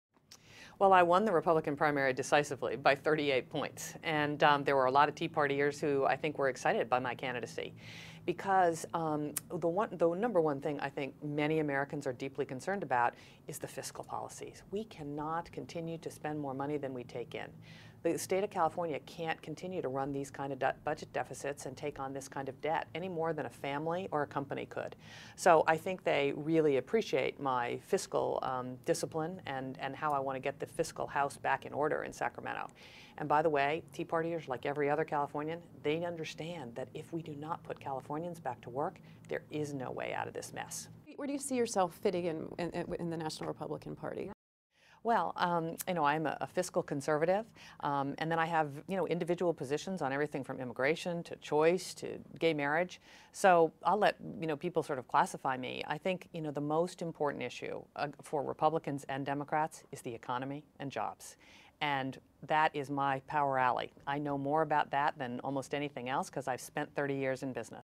Meg Whitman: A Lot of Tea Partiers Were Excited by My Candidacy
Republican California gubernatorial candidate Meg Whitman talks about her approach to fiscal discipline, her support from the tea party, and her campaign's focus on jobs and the economy.